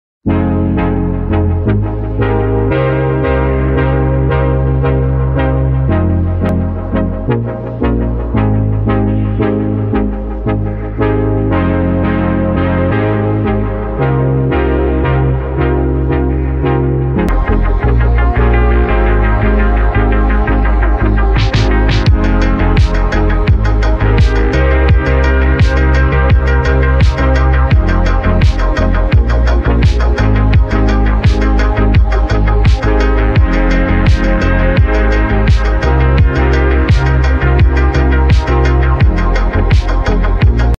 Green Day old images colourized [SUB FOR PART 2]
Using an AI I managed to turn old black and white photos of punk/rock bands into coloured images